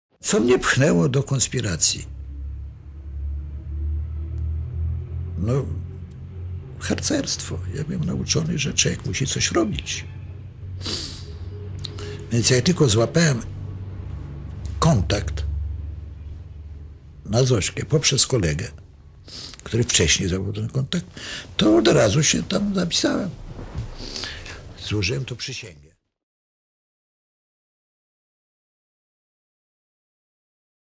„Do konspiracji pchnęło mnie harcerstwo” | 80. rocznica powstania AK
„Do konspiracji pchnęło mnie harcerstwo. Byłem nauczony, że człowiek musi coś robić” (Stanisław Krupa „Nita”, kompania „Maciek”, baon AK „Zośka”).